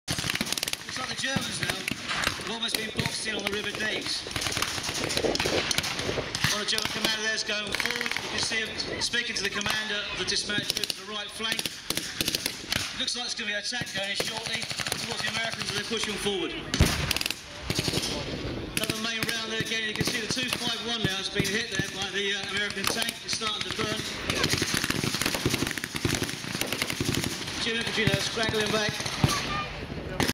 Wehrmacht and Waffen XX Soldiers Reenactment at War and Peace Show Beltring 2010
Beachtet auch meine anderen Videos / Have a look at my other Videos also!